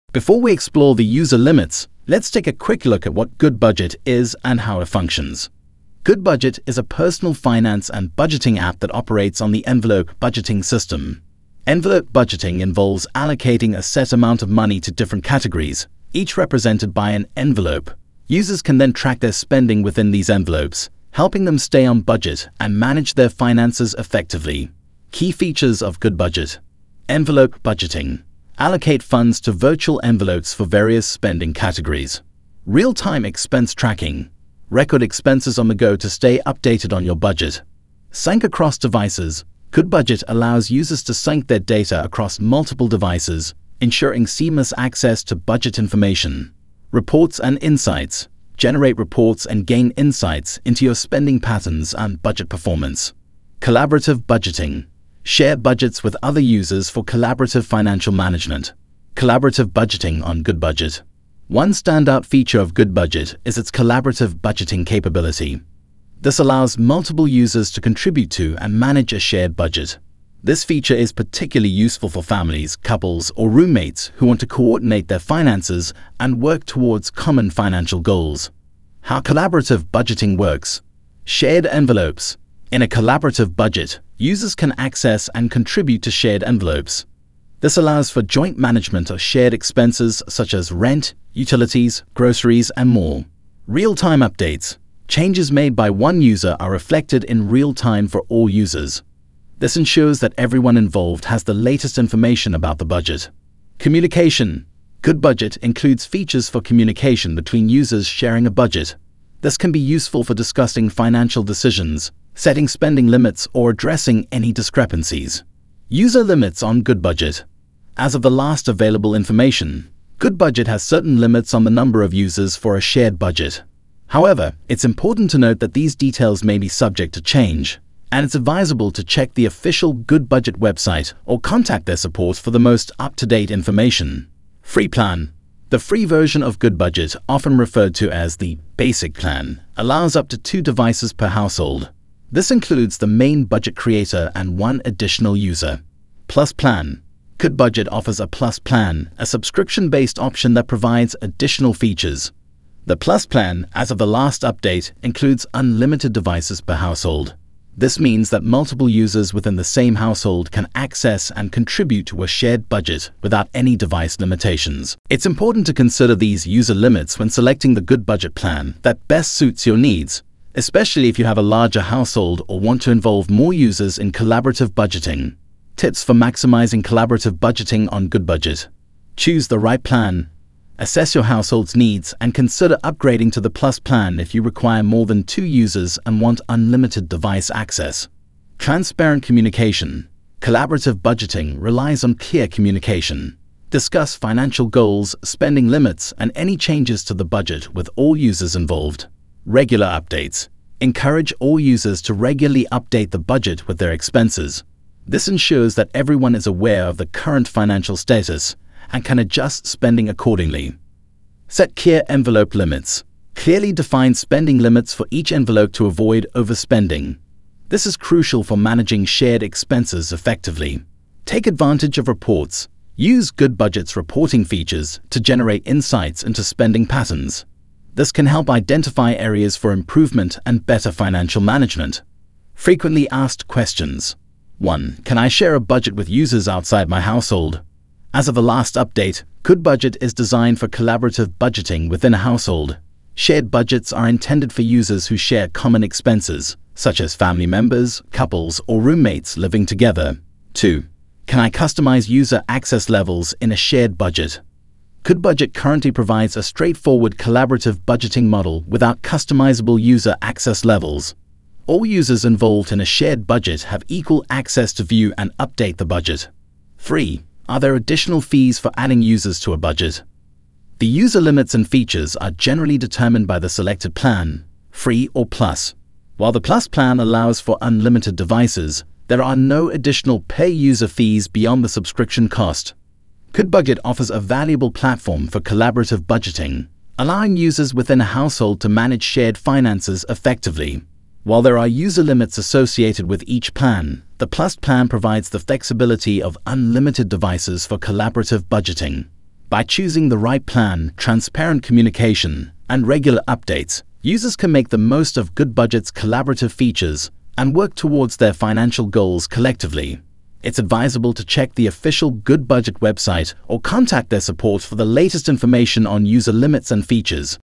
How many users can you have on Goodbudget?
Goodbudget, a popular budgeting app, has gained recognition for its user-friendly interface and effective budget management features.